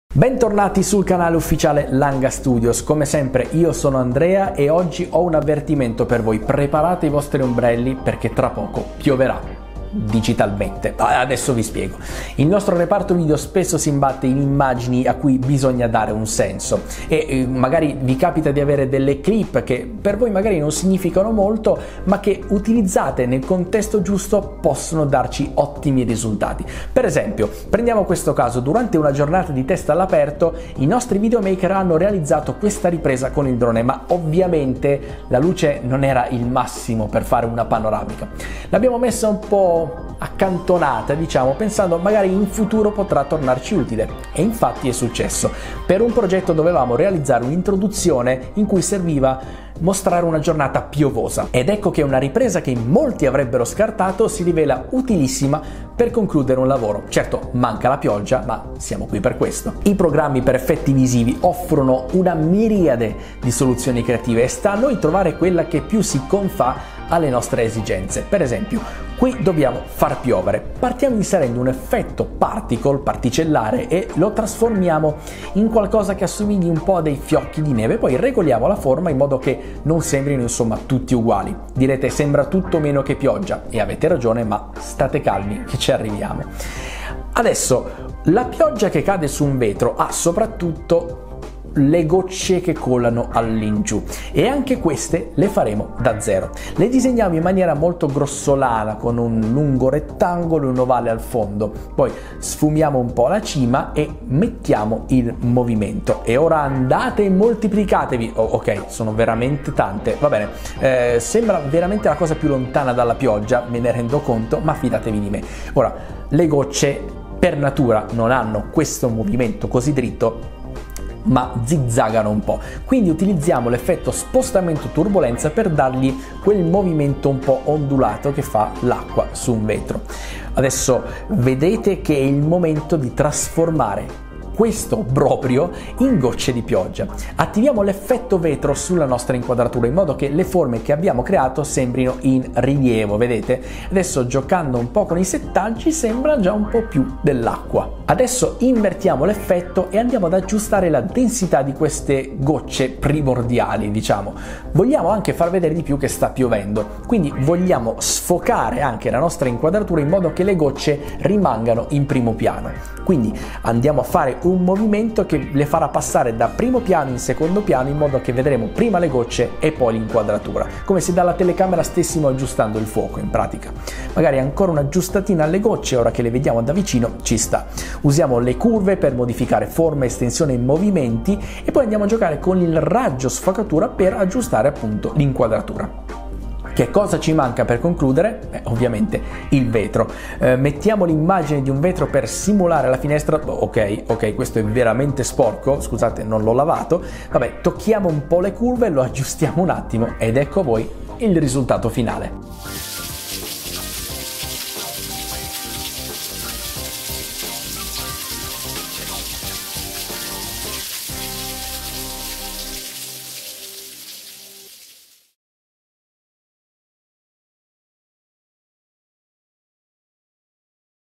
Creazione EFFETTO GOCCE DI PIOGGIA su vetro DA ZERO
Il nostro reparto video spesso si imbatte in immagini a cui bisogna dare un senso. Possono essere inquadrature di pochi secondi, se prese da sole magari non dicono niente, e invece con gli effetti giusti possono tornarci molto utili. Prendiamo questo caso. Durante una giornata di test all’aperto, abbiamo realizzato questa ripresa con il drone. Ma ovviamente la luce non era il massimo per una panoramica. Ce lo siamo tenuto un po’ da parte, chi lo sa, può sempre tornare utile, e infatti in una lavorazione serviva un’introduzione che mostrasse una giornata piovosa.

👇 SCOPRI CHE COSA SPIEGHIAMO IN QUESTO VIDEO 👇
00:00 - Introduzione al progetto
00:28 - Utilizzo inaspettato della ripresa con il drone
00:55 - Creazione dell'effetto pioggia con particelle
01:22 - Aggiunta del movimento alle gocce di pioggia
02:29 - Rifinitura e aggiustamenti finali